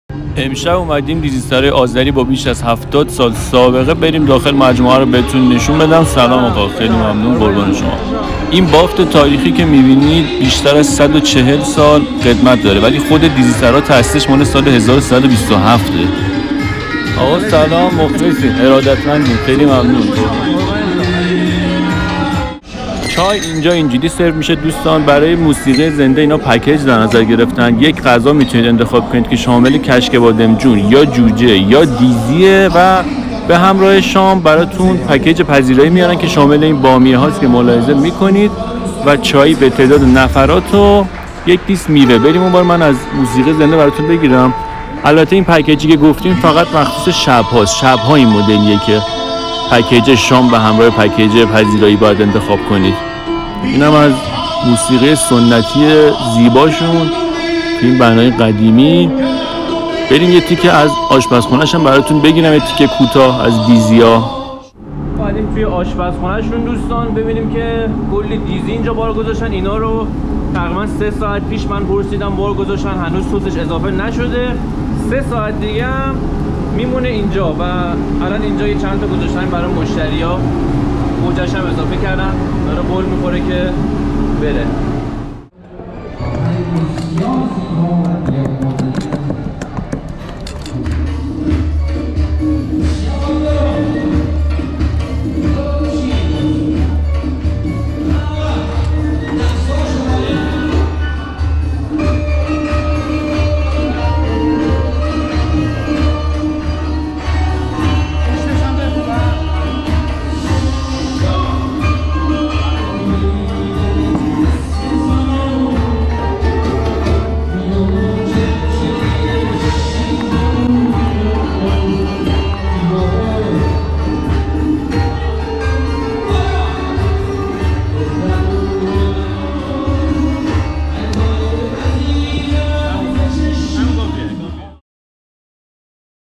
دیزی سرای سنتی آذری راه آهن
دیزی سرای سنتی آذری راه آهن با بیش از هفتاد سال سابقه. دیزی خورا اینجا رو میشناسن و نیازی به معرفی نداره.از قدیمی ترین هاست و جزو آثار ملی هم ثبت شده. دیزی هاشم محشره خیلی خوشمزه هست اگه نرفتید حتما برید هم از فضاش و هم از غذاش لذت ببرید.
شبها موسیقی زنده سنتی و آذری عالی هم داره.
پکیج پذیرایی به این صورت برای شام سه تا انتخاب دارید: یا دیزی یا جوجه یا کشک بادمجون به همراهشون هم مخلفات مثل سبزی ...و بعدش هم دیس میوه و چایی و بامیه خیلی مزه میده.
آدرس: میدان راه آهن ابتدای خ ولیعصر پلاک سی